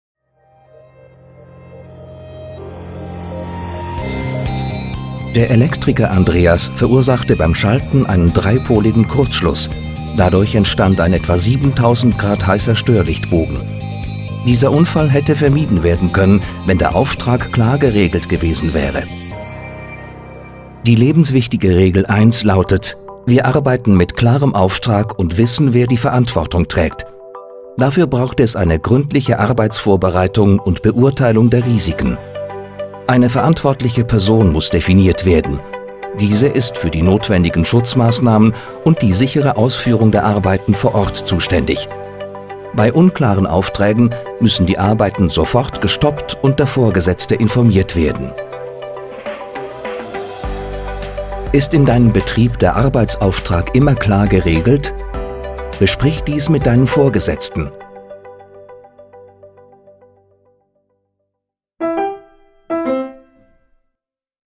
Lebenswichtige Regel 1 für Betriebe der Energieerzeugung und -versorgung
Instruktionsfilm für Elektrofachleute. Nutzen Sie dieses Video für die Instruktion der 5 + 5 lebenswichtigen Regeln im Umgang mit Elektrizität. Regel 1: Wir arbeiten mit klarem Auftrag und wissen, wer die Verantwortung trägt.

Die Suva ist mehr als eine Versicherung: sie vereint Prävention, Versicherung und Rehabilitation.